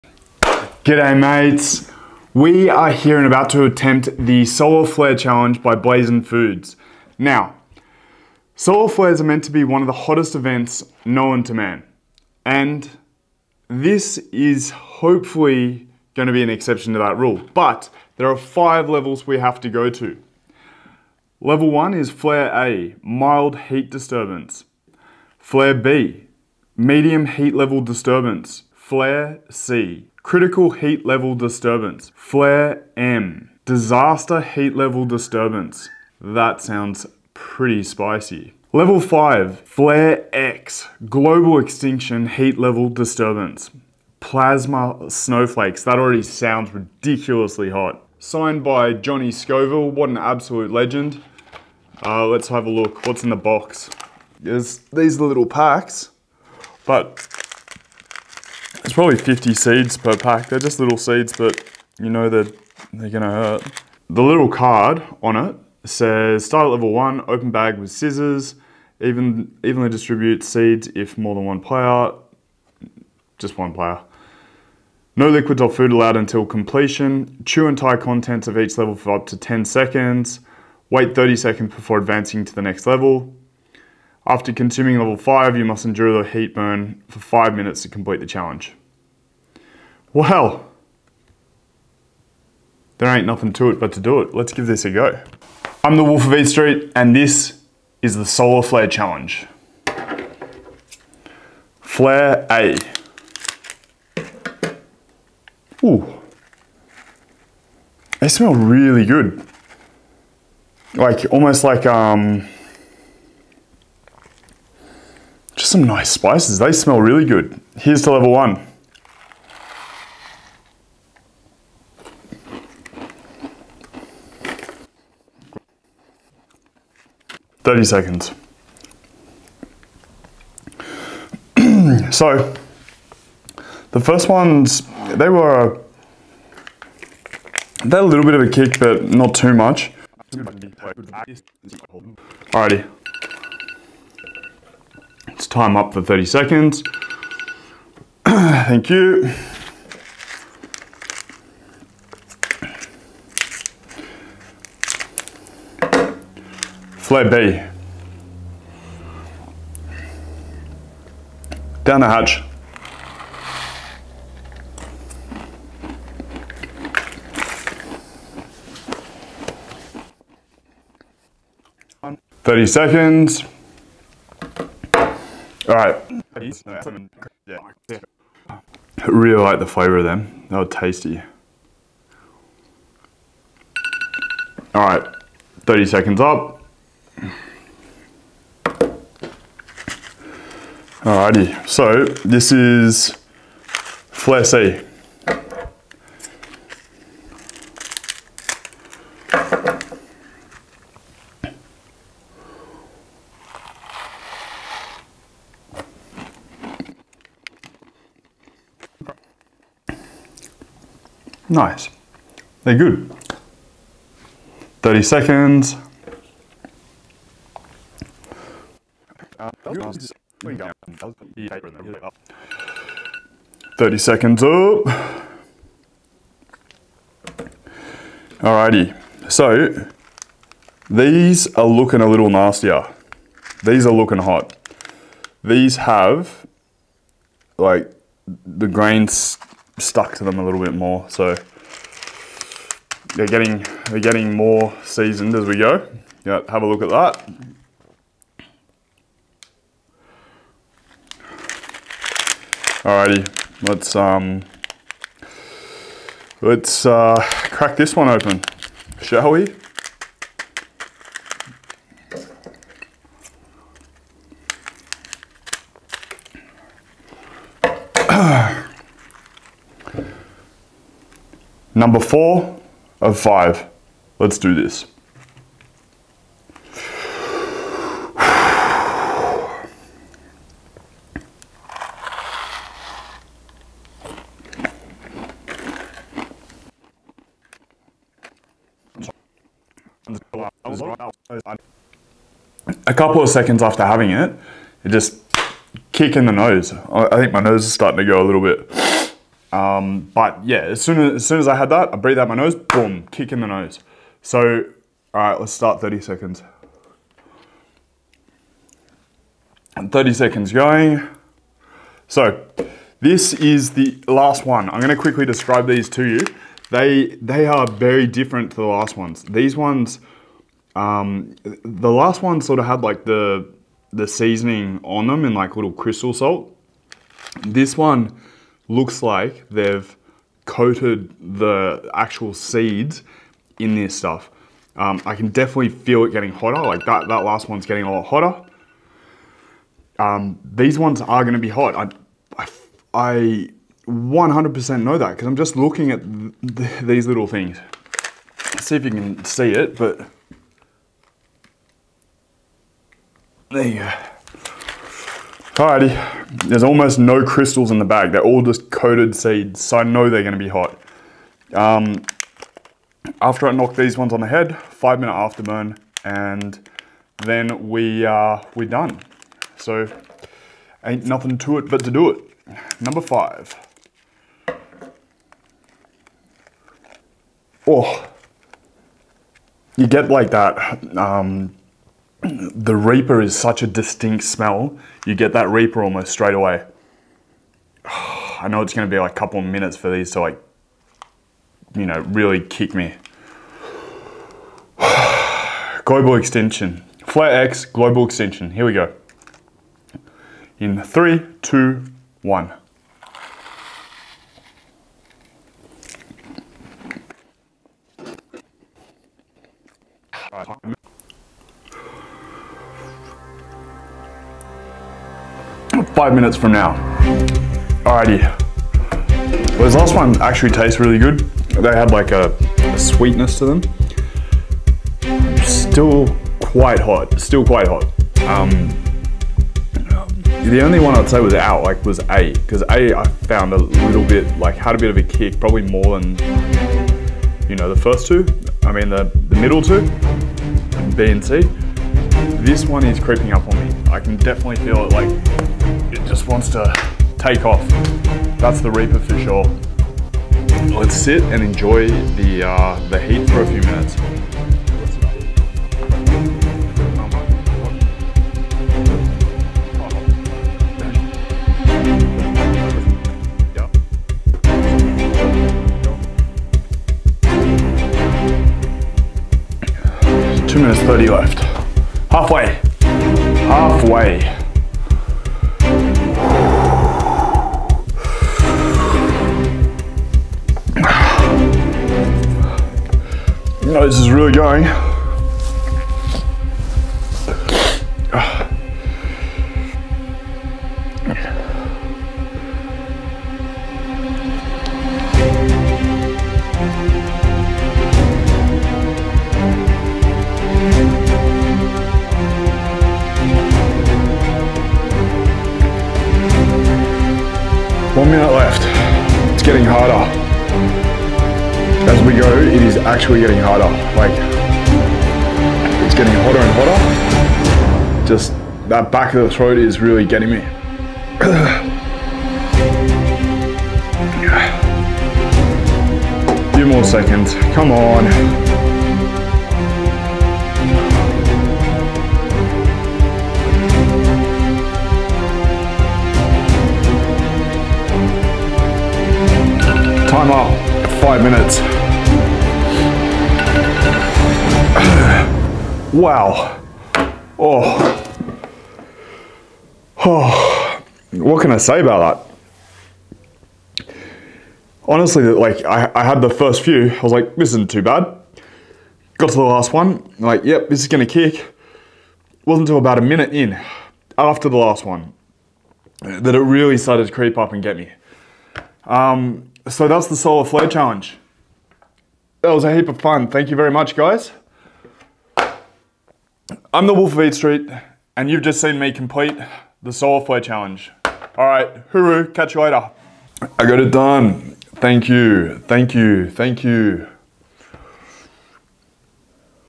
THE WORLD'S HOTTEST SUNFLOWER SEEDS | SOLAR FLARE SUNFLOWER SEED CHALLENGE | TheWolfOfEatStreet
THIS ONE HURT!

From the team that brought you Death Nuts, today I attempted the Solar Flare Challenge, AKA the World's Hottest Sunflower Seeds!

The rules of the challenge are:
- No liquids or anything else whilst attempting the challenge
- 30 seconds of burn time between levels
- 5 minutes of burn time after Level 5
- All seeds must be chewed

By the way, how cool is the packaging?! and the legendary Johnny Scoville signed the box!

Let me know what you think and drop a comment!

From their site:
Solar Flares are some of the hottest events recorded in our Universe and are classified into 5 major levels of intensity – A, B, C, M & X. Our Solar Flare Sunflower Seed Challenge is classified exactly the same way…

Level 1 – Flare A: this one is a MILD heat level disturbance with Habanero & Scotch Bonnet Peppers with Mild Red Chili’s & Lime.

Level 2 – Flare B: this one is a MEDIUM heat level disturbance with Red Savina Peppers, Adobo Seasoning & Cilantro.

Level 3 – Flare C: this one is a CRITICAL heat level disturbance with Ghost Pepper, Devil’s Breath Pepper, 7-Pot Brain Strain Peppers, Onion & Garlic.

Level 4 – Flare M: this one is a DISASTER heat level disturbance with Trinidad Moruga Scorpion Pepper and a Tunisian Harrisa Pepper Spice Blend.

Level 5 – Flare X: this one is a GLOBAL EXTINCTION heat level disturbance – Plasma Snowflakes with a painful blend of Carolina Reaper Pepper, Trinidad Moruga Scorpion Pepper, 7-Pot Douglah, and a blend of savory spices.